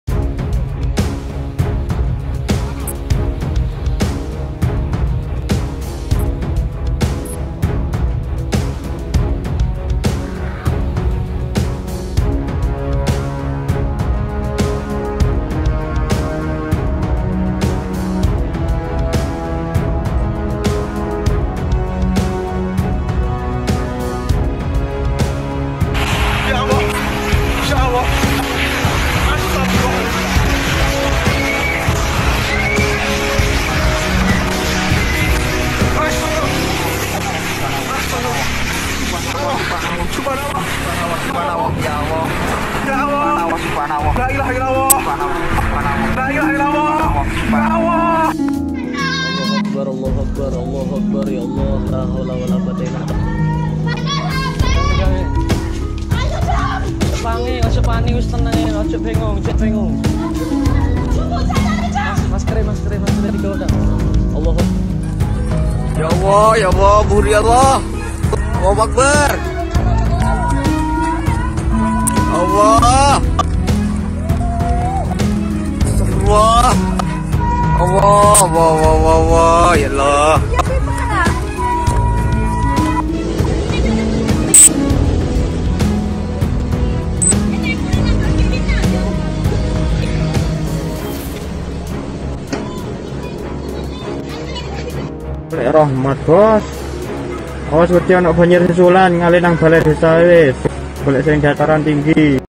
Terekam dengan jelas,video amatir warga gunung Semeru erupsi.
letusan gunung Semeru